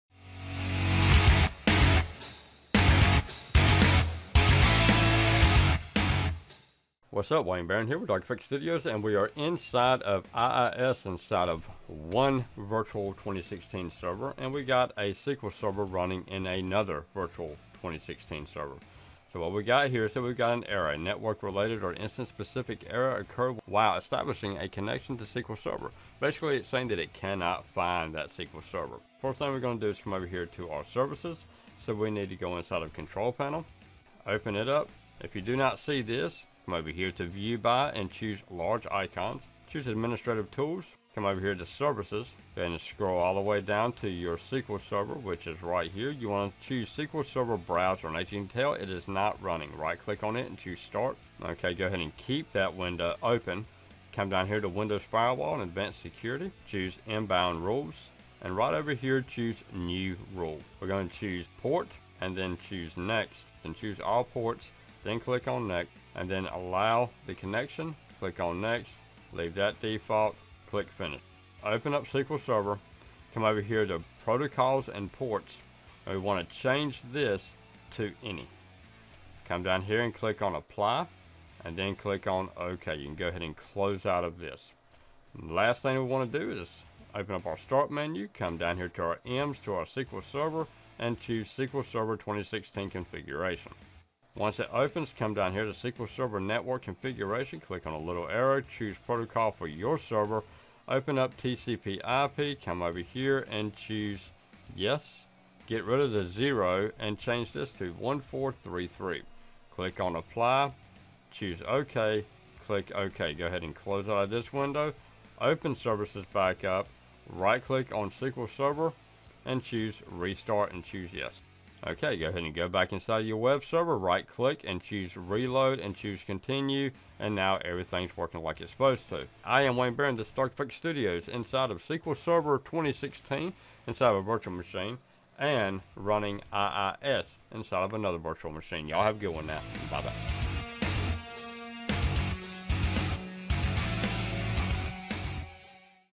SQL Network Interfaces error 26 Error locating server/instance specified
In this lesson we are going to correct some settings, to allow our SQL Server to be connected to from another computer.

Full Error.
A network-related or instance-specific error occurred while establishing a connection to SQL Server. The server was not found or was not accessible. Verify that the instance name is correct and that sql server is configured to allow remote connections. (Provider: SQL Network Interfaces, error: 26 - Error Locating Server/Instance Specified)

The microphone
Audio-Technica AT2020USB Plus Condenser Microphone
UK Customers

UK Customers